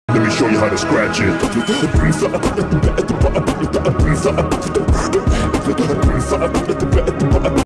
[FNAF SFM] Springtrap Shows Ya How To Scratch It [Green Screen]
Just thought I'd create this here animation & green screen & see if folks'll use it. If so, make sure to credit me as the original source, y'all! Have a blessed one now.